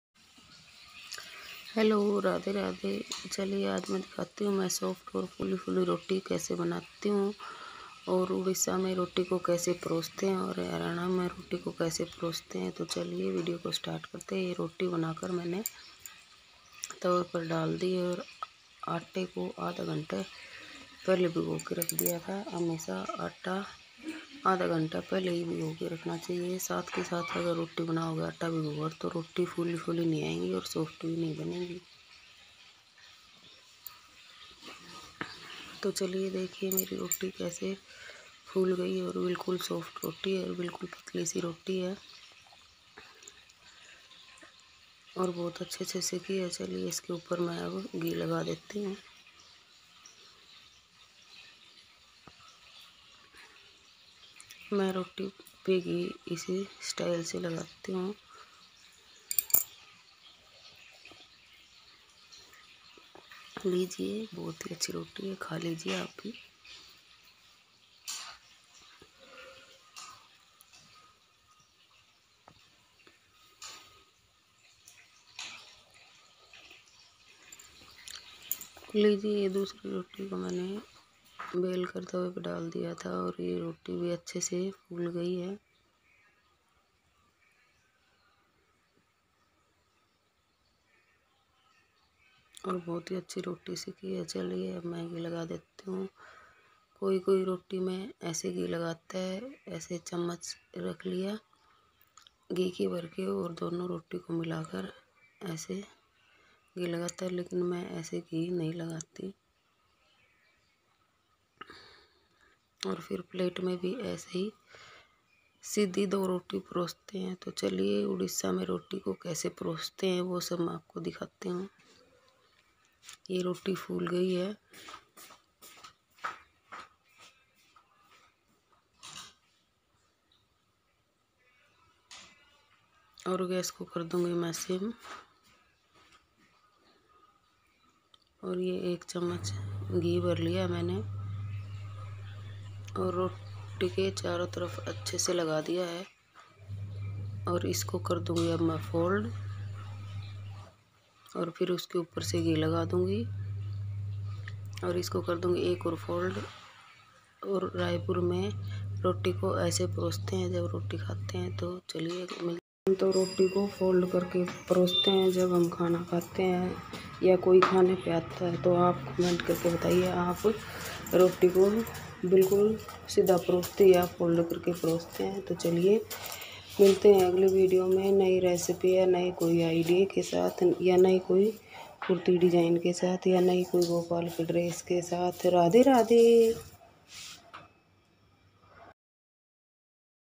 फरमाइशी विडियो हम रोटी को ऐसे परोसते हैं हमारे कोई खाने पर आते हैं तो आप के यहां कैसे परोसते हैं सब
Roti_ki_Recipr_. khatu shyam Vlogs me apka dil se swagat hai all